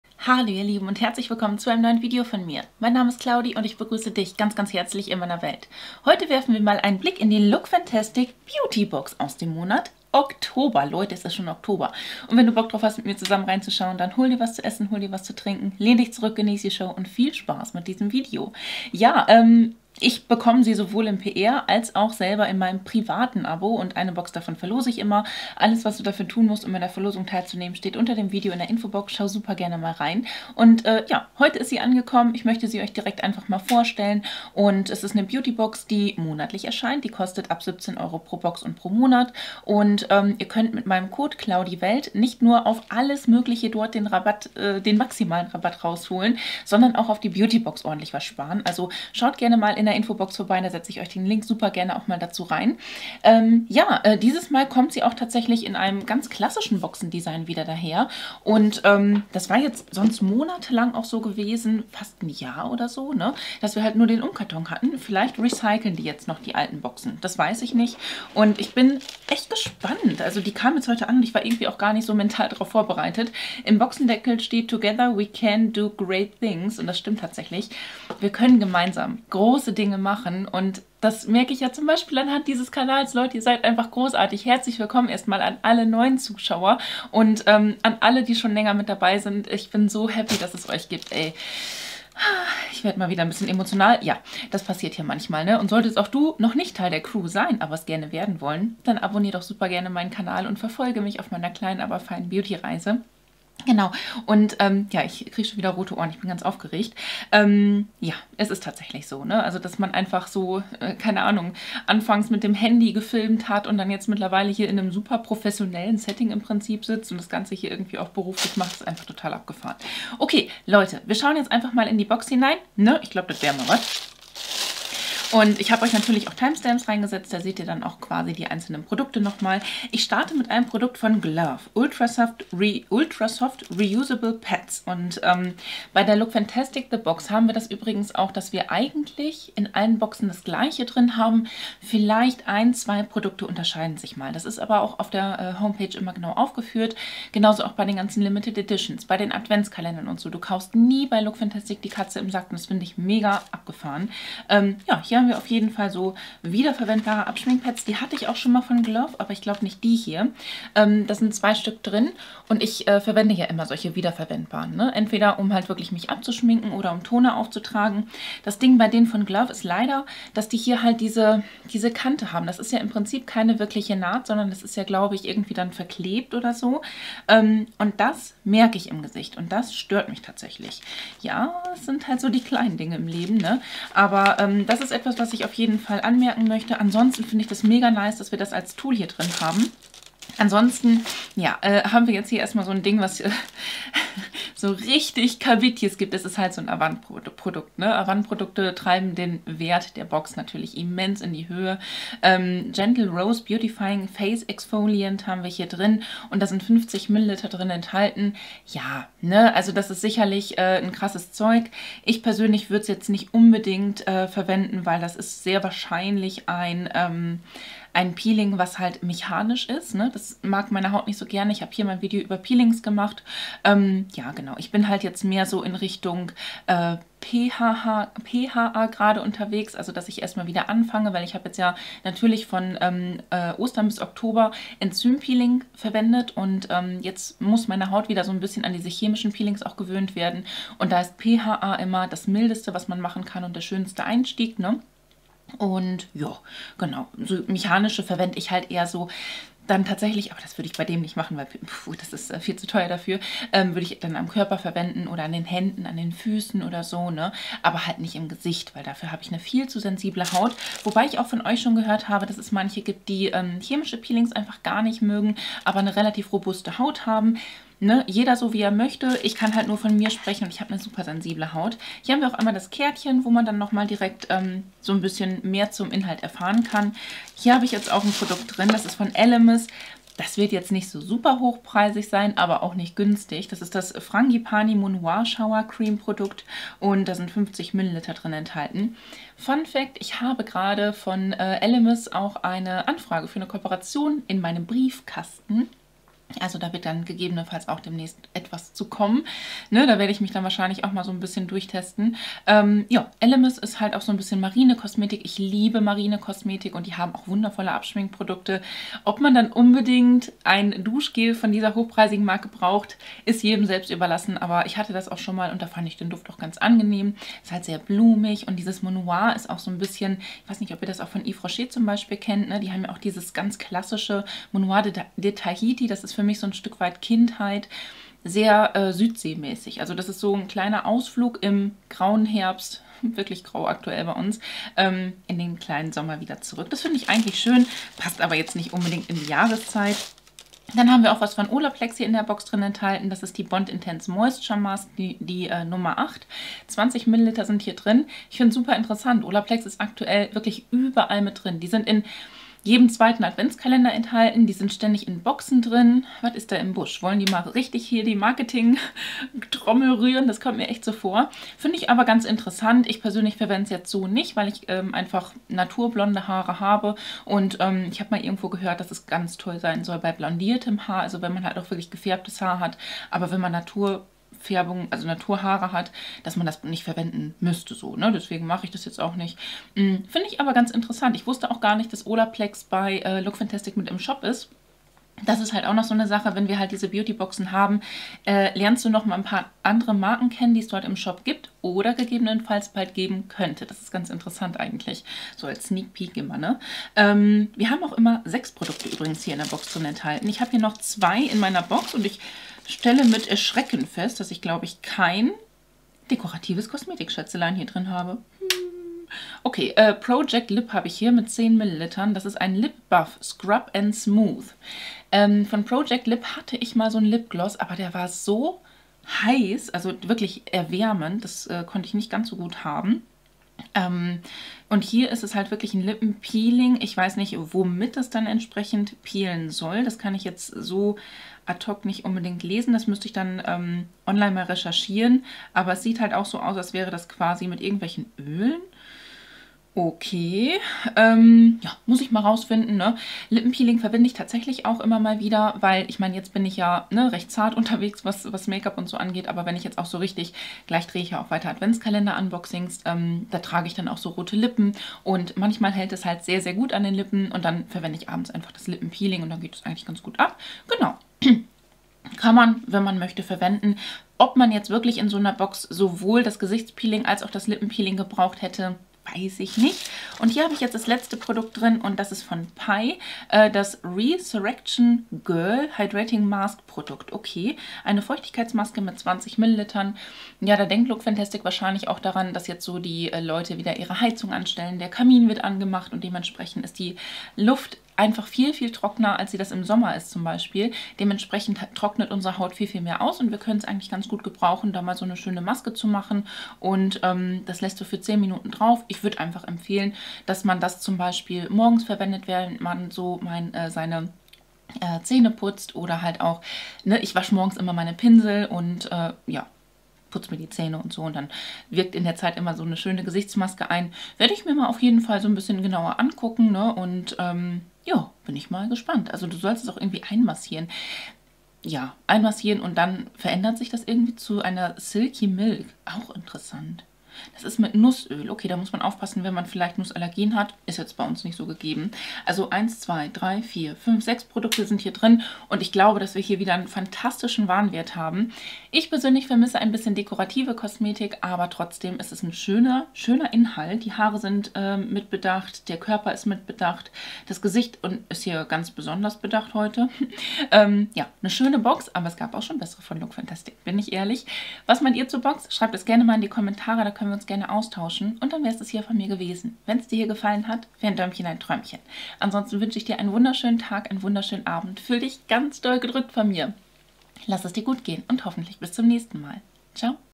Lookfantastic The Box Oktober 2024 Inhalt & Verlosung
die lookfantasticbeautybox erscheint monatlich und kostet ab 17 € pro Box und Monat.
Sie enthält 6 Beautyprodukte und einen deutlich höheren Warenwert, als das, was wir für die Box bezahlen.
Im oktober erhalten wir ausschließlich Pflege Produkte, die einen immensen Warenwert ausrufen.

Vielen Dank an das @lookfantastic  Team für die Zusendung der Box!
Meine eigene Abobox verlose ich in diesem Video.

MEIN NEUES BUCH: "Vergangene Verse" & Mein 5-Minuten-Tagebuch sind jetzt im Peletierverlag verfügbar :)

00:00 Infos zu Lookfantastic The Box
Die Box erscheint monatlich und kostet ab 17 € pro Box
LOOKFANTASTIC Trending Beauty Edit (L,*)
LOOKANTASTIC The Scent Edit Edit (L,*)
02:10 GLOV wiederverwendbare Pads
03:10 Avant Skincare Gentle Rose
04:37 ELEMIS Frangipani Monoi Duschcreme
05:41 Olaplex No.8 Bond
06:38 Project Lip Lip Buff
07:50 Pai Skincare Resurrection Girl
08:52 Mein Fazit

♡ Schreibe mir einen lieben Kommentar unter dieses Video mit der Info, welches dein Lieblingsprodukt aus der Box ist
♡ Der Gewinner wird per Antwort auf ihren/seinen Kommentar benachrichtigt und muss sich innerhalb einer Woche nach Benachrichtigung bei mir per Mail oder Instagram melden

Liebe Grüße
Claudi